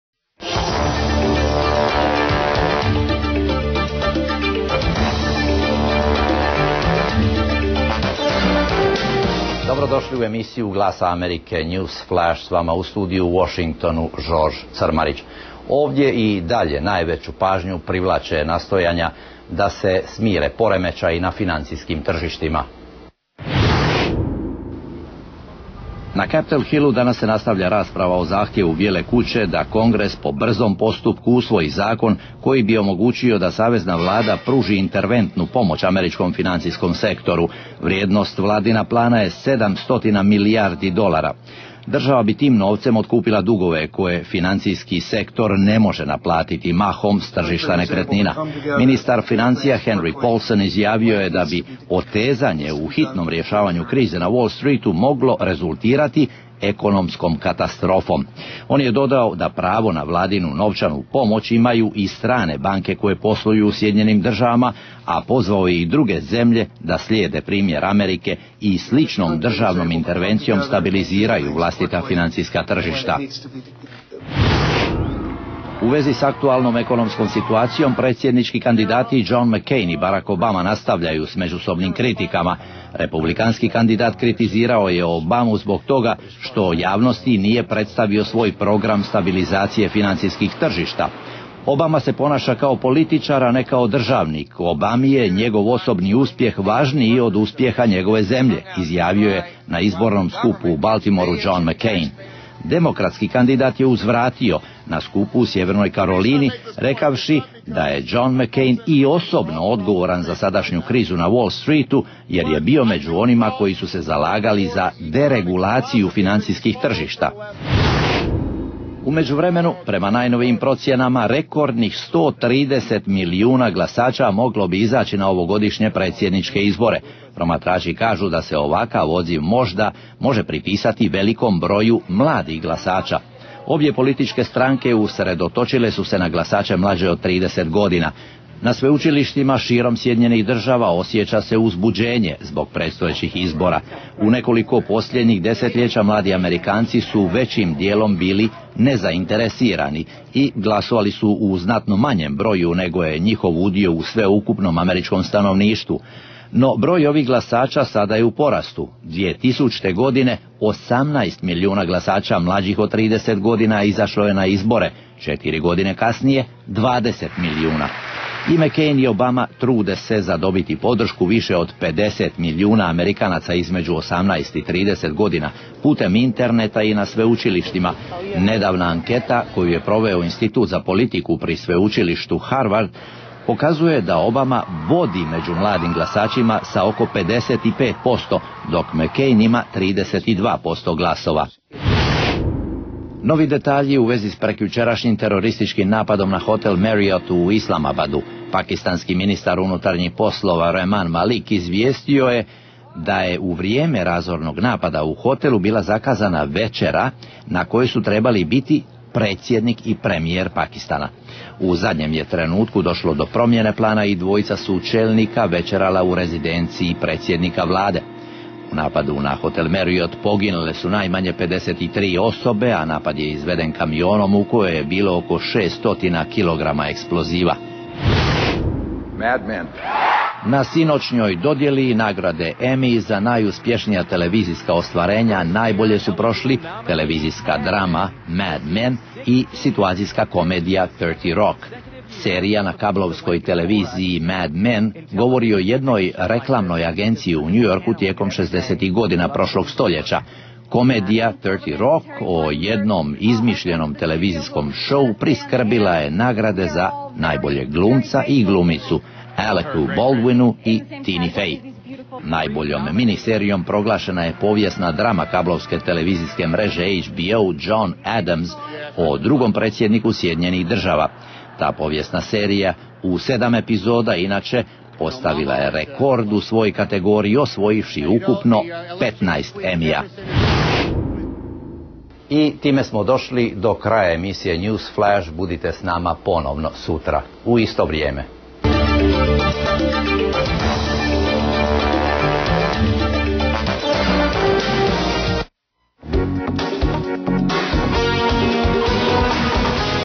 NewsFlash 22. rujna 2008.
Pripremili Žorž Crmarić i Andrea Walsh. Kongres treba raspraviti odobrenje 700 milijardi dolara za financijske intervencije. Barack Obama i John McCain privlače mlade birače.